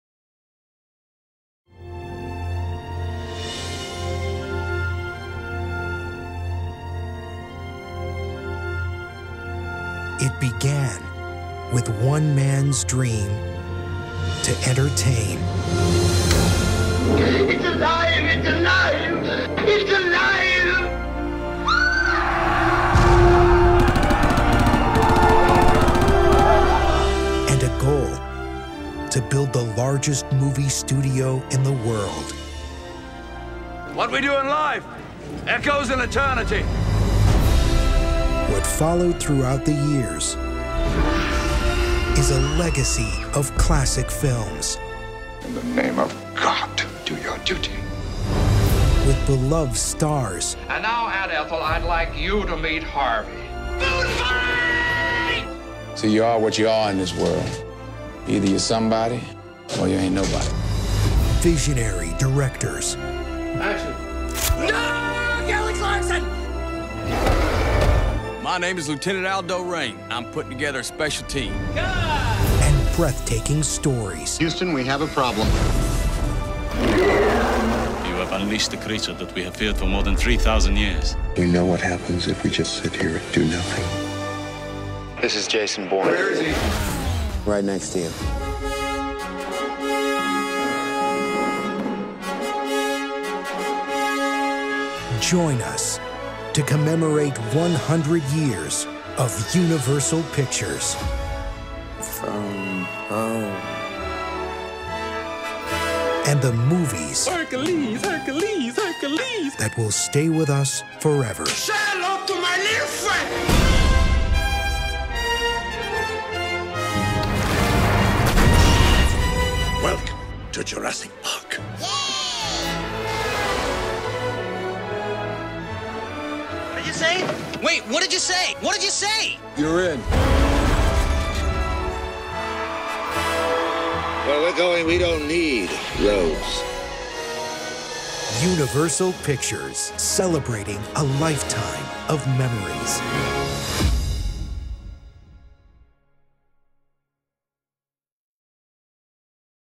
Universal 100th Anniversary - Trailer
This year we're celebrating our 100th anniversary, and you can get involved on facebook! With new films, classic DVDs, essential Blu-Rays, events, online games, competitions, and money-can't-buy prizes.